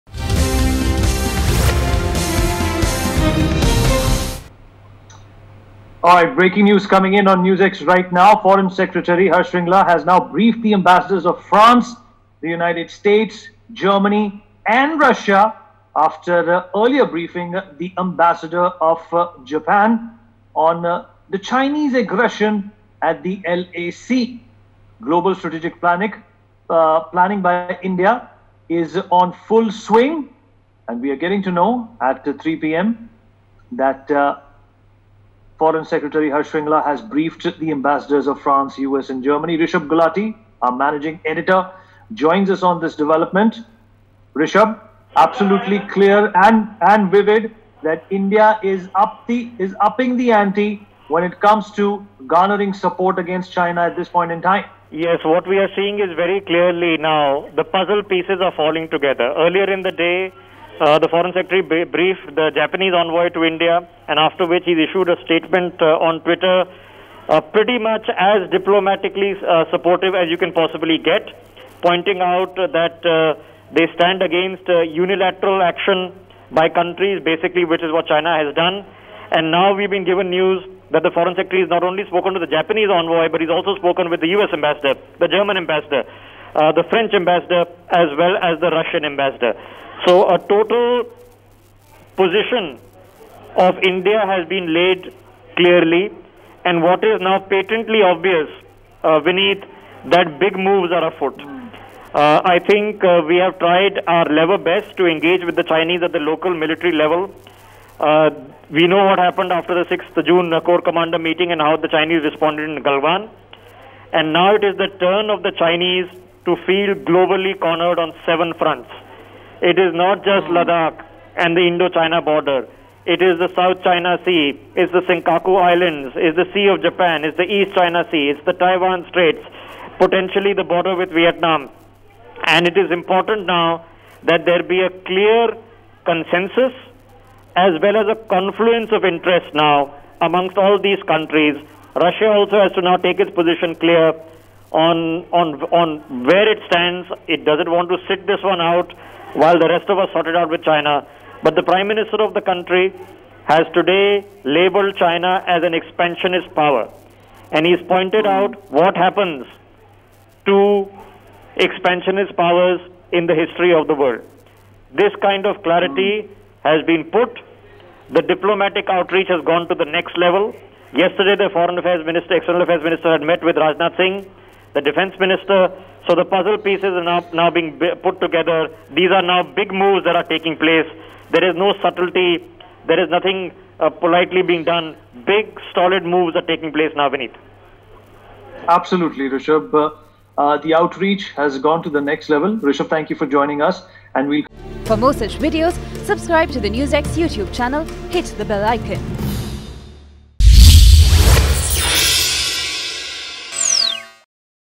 India Briefs France, U.S & Germany | China Being Cornered | NewsX
India's Foreign Secretary has briefed U.S, France, Germany ambassadors. FS Shringla also briefed the envoy of Russia on the Chinese aggression at LAC. Now, the global isolation of China is underway. Meanwhile, most of the Powers have backed India's position. | NewsX